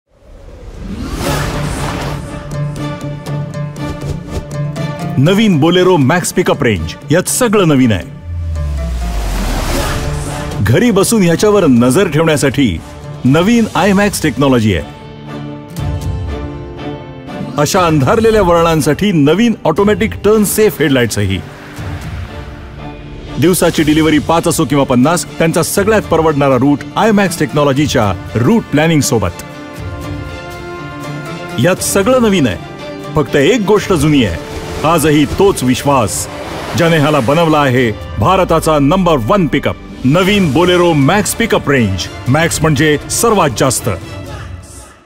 नवीन बोलेरो मॅक्स पिक-अप रेंज. मॅक्स टेक्नोलॉजी आणि मॅक्स सेफ्टी
आता सादर करत आहोत नवीन बोलेरो मॅक्स पिक-अप रेंज। ह्यात तुम्हाला iMaxx टेक्नोलॉजी सोबत रूट प्लॅनिंग आणि वेहिकल ट्रॅकिंगही मिळेल. त्यासोबत अंधार असलेल्या वळणावर संरक्षण देणारी ऑटोमॅटिक टर्न सेफ हेडलाइट्ससुद्धा. ह्यात खूप काही नवीन आणि अपेक्षेपेक्षा जास्त आहे. कारण मॅक्सचं प्रत्येक काम आहे - सर्वात जास्त!